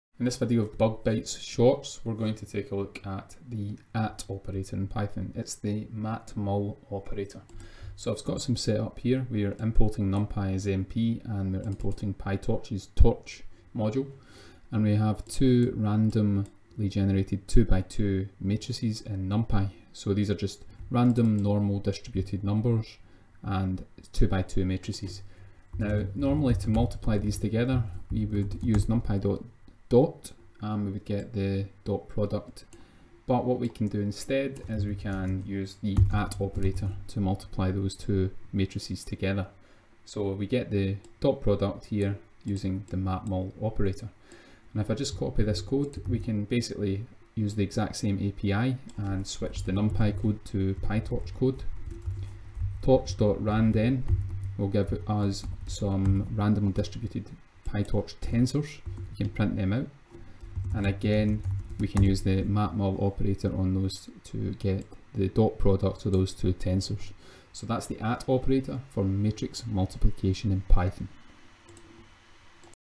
Python Shorts - @ operator for matrix multiplication in Numpy and PyTorch
In this Python shorts video, we take a look at the @ operator for matrix multiplication, introduced in Python 3.5 as a result of PEP-465

In the video, we demonstrate the matrix multiplication of two Numpy arrays, and two PyTorch tensors.